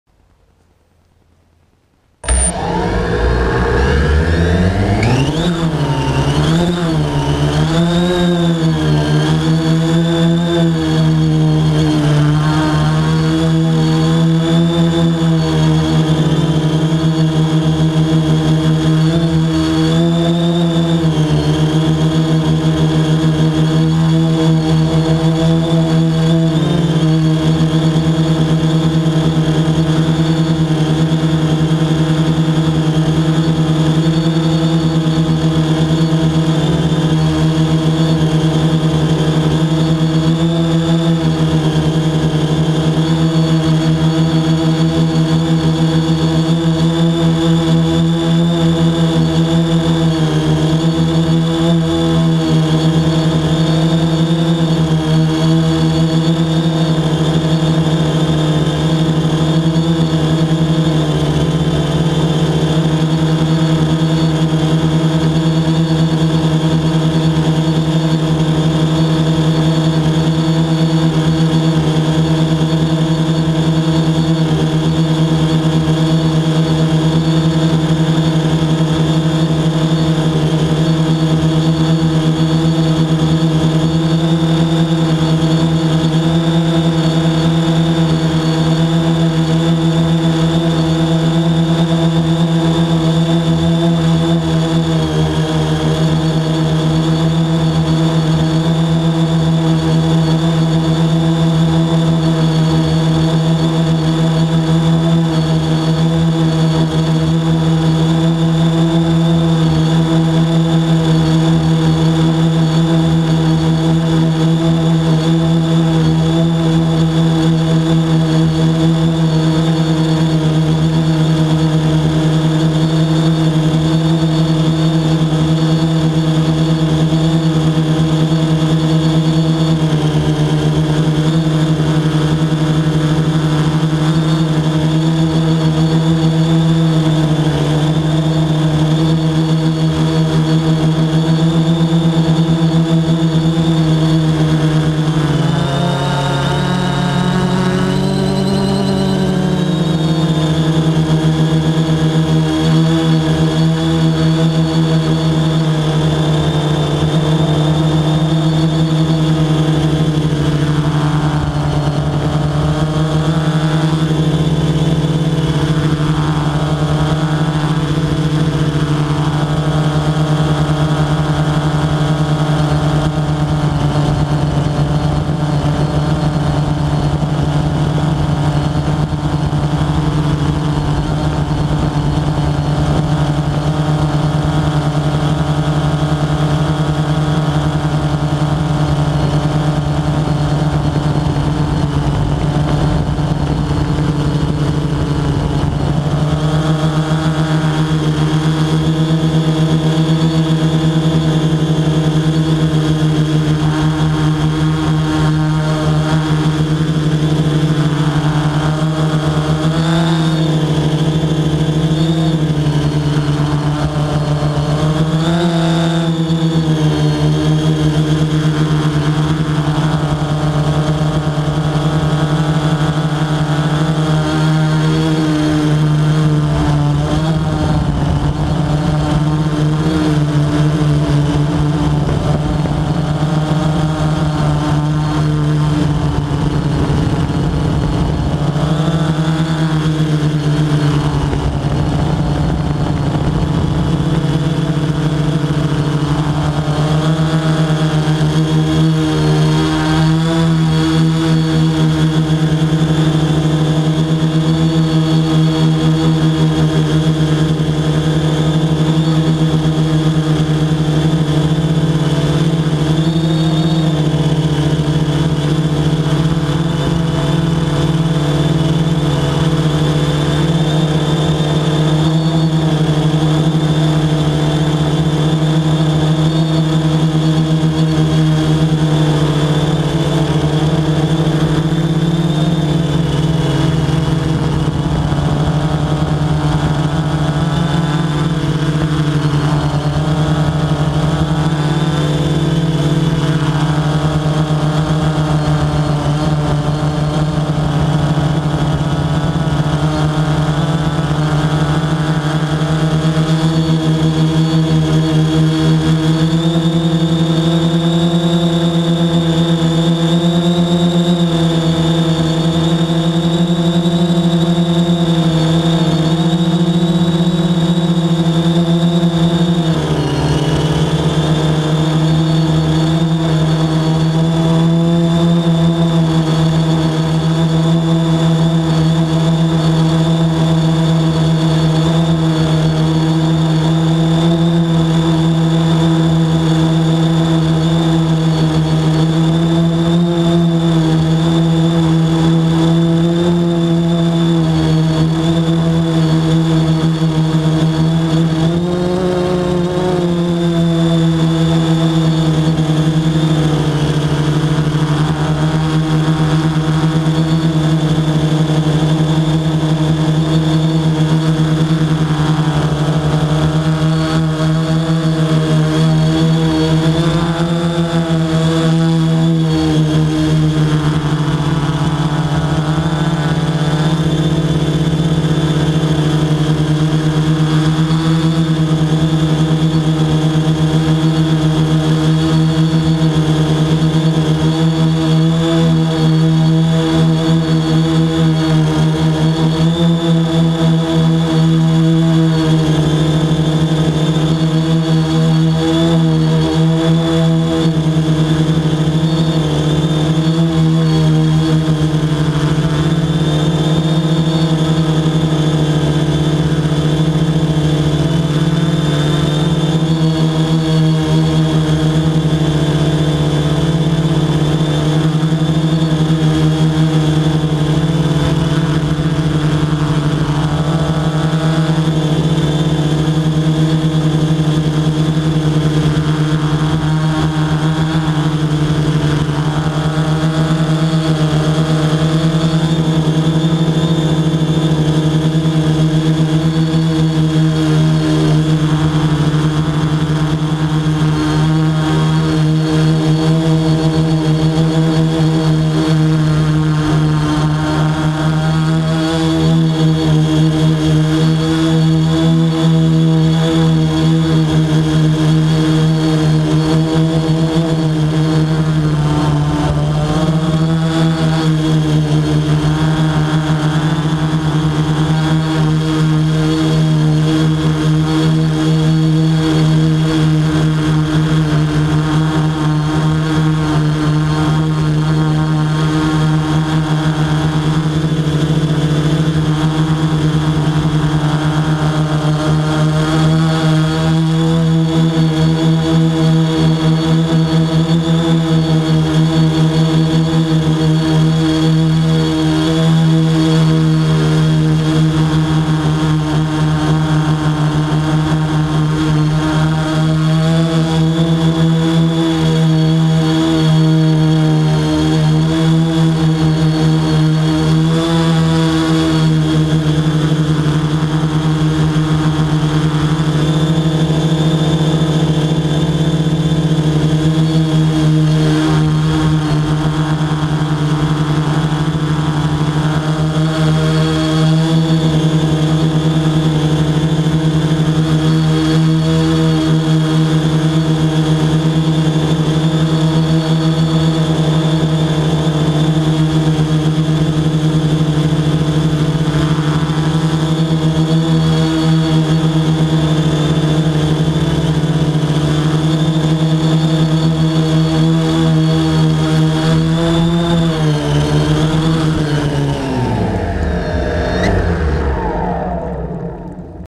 Supermoon over Owensboro KY
Up above the skyline with "Larry" looking at the Supermoon.